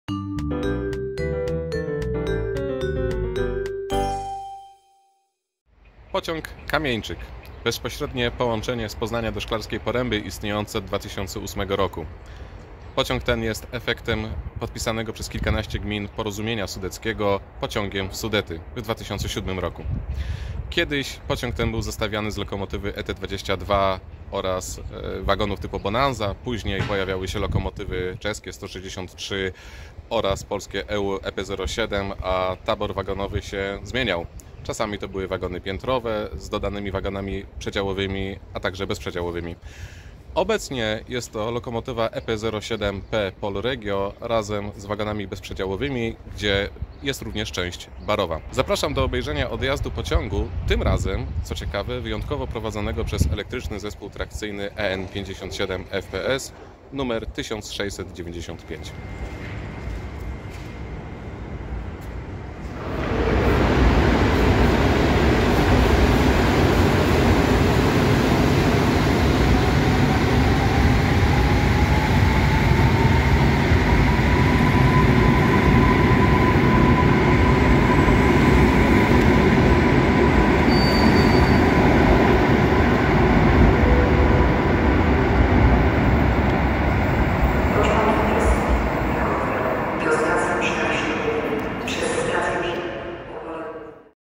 Pociąg Kończy Bieg [#221] Polregio R 76108 "Kamieńczyk" - EN57FPS-1695
Pociąg Kończy Bieg - codzienne wideo z pociągiem o 16:00 w dni powszednie - w sam raz na koniec dnia!

Wrocław Główny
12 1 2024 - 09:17

Polregio R 76108 "Kamieńczyk"
Poznań Główny - Szklarska Poręba Górna
EN57FPS-1695